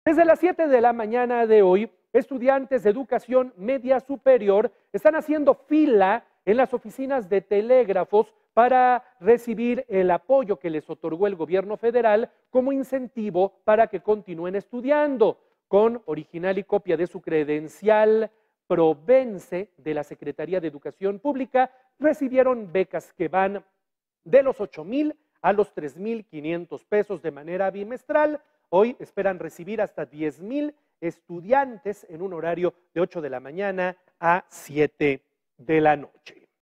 Filas para recibir apoyo || Noticias con Juan Carlos Valerio
Estudiantes de educación media superior hicieron largas filas para recibir el apoyo que les otorgó el Gobierno Federal como incentivo para que continúen estudiando.